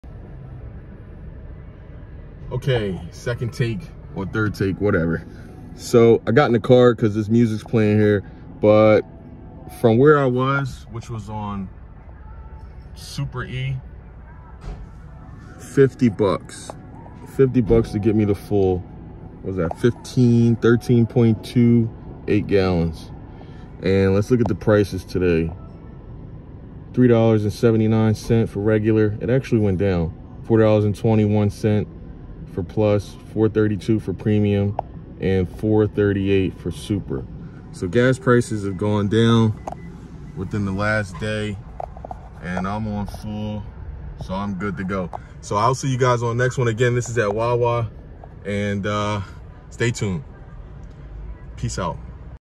TODAY'S GASOLINE PRICES 10/22/22
Today it appears the fuel prices has gone down about 15-20 cent. I am reporting in the Philadelphia Metro Area.